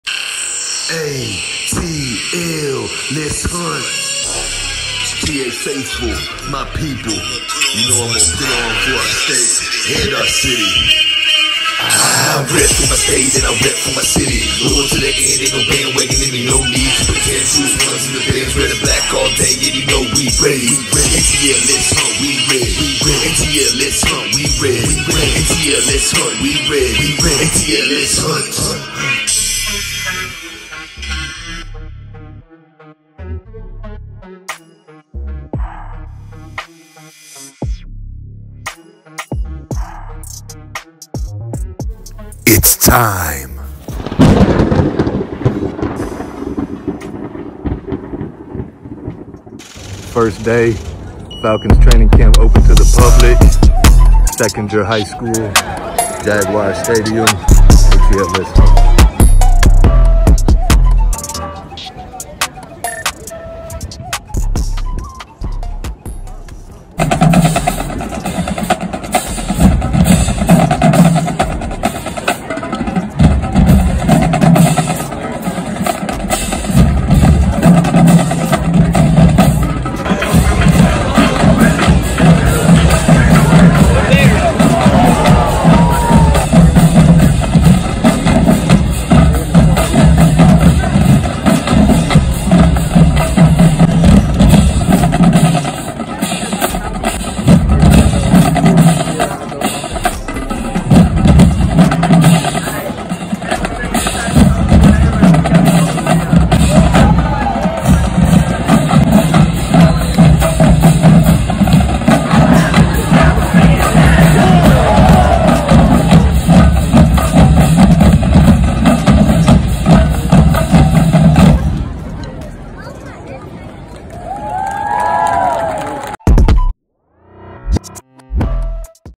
Atlanta Falcons Training Camp Live Inside Action
Step into live view of ATL FALCONS training camp LIVE!!! Much more to come.